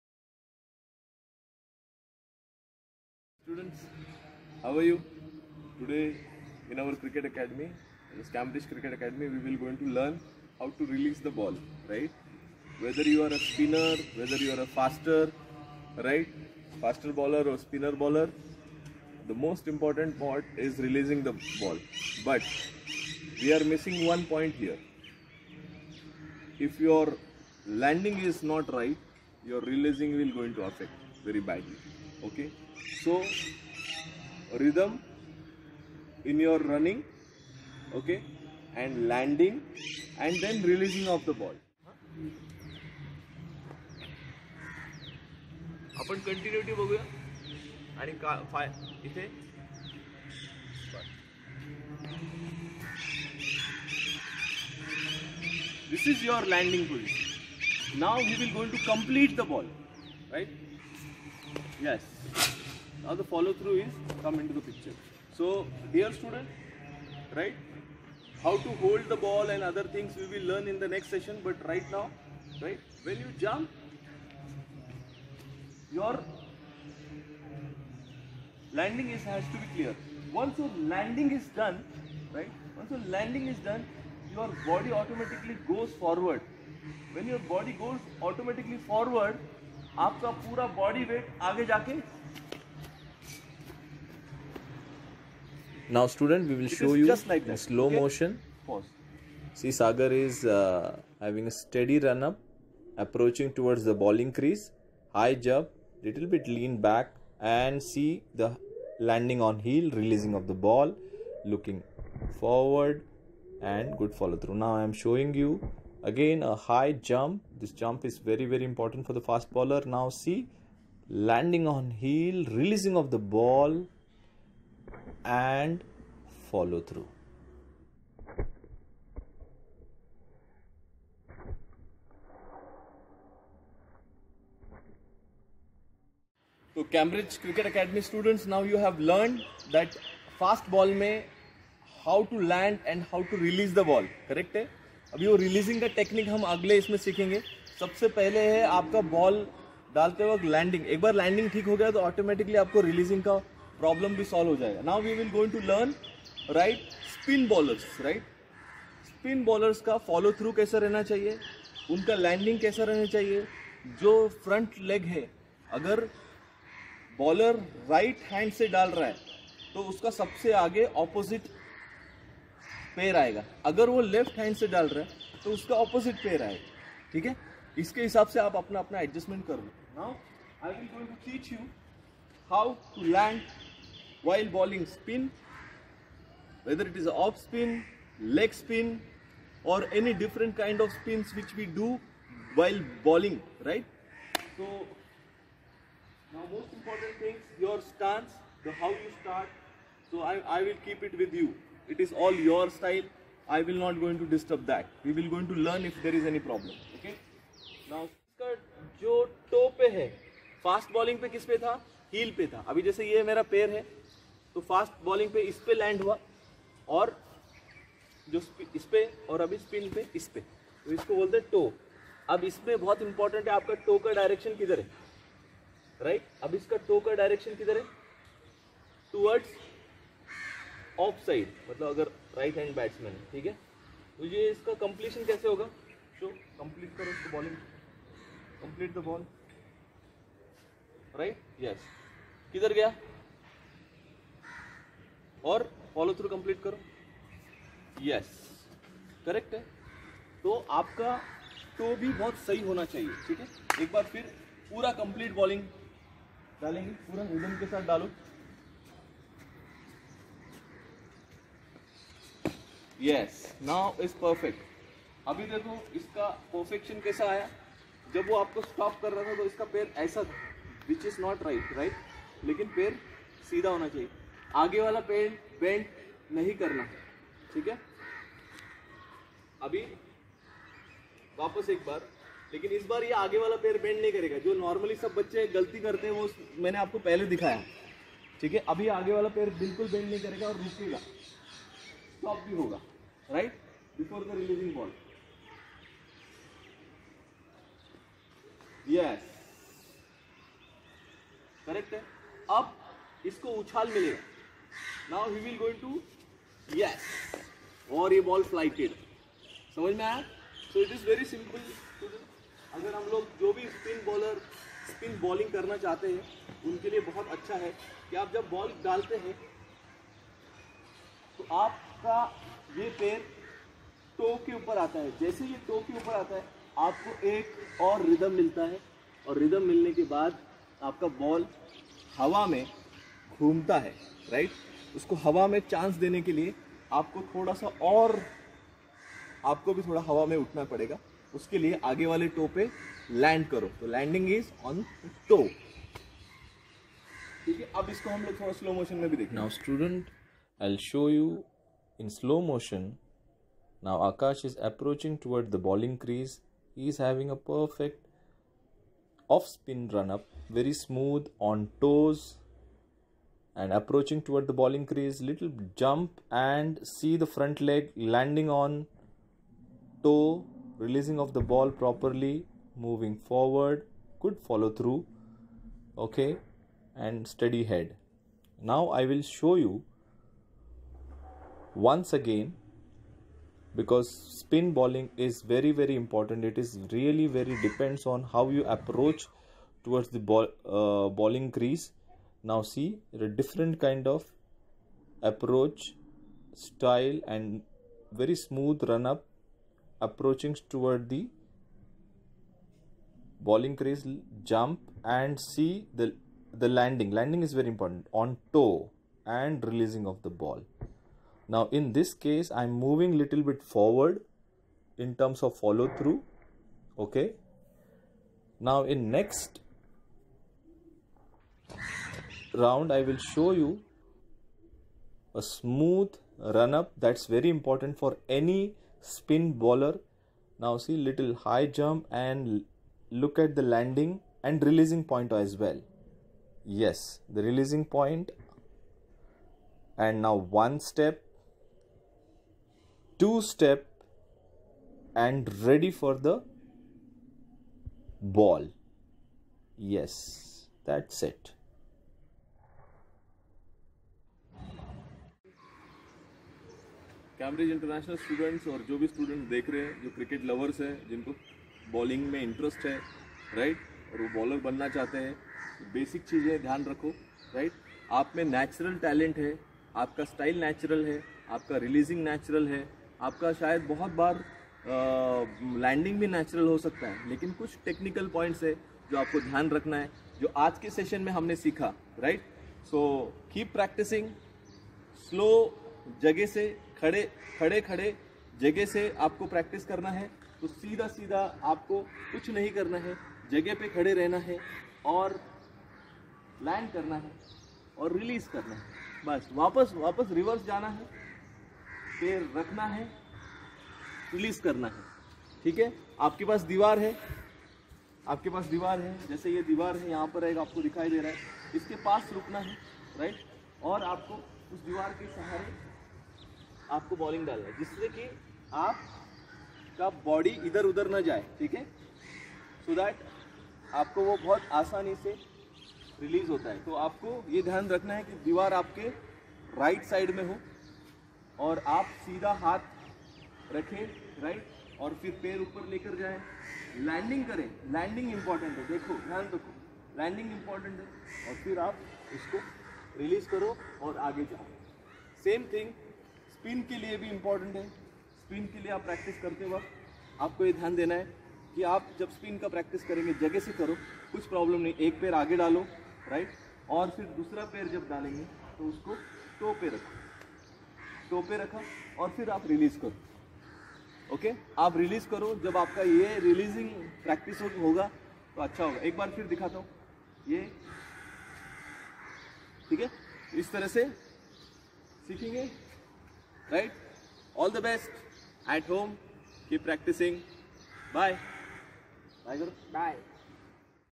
Cambridge cricket academy Basic Bowling Techniques , By Ram D. Raina
Faster Bowler should land your front leg on Heel and spinners should Land on Toe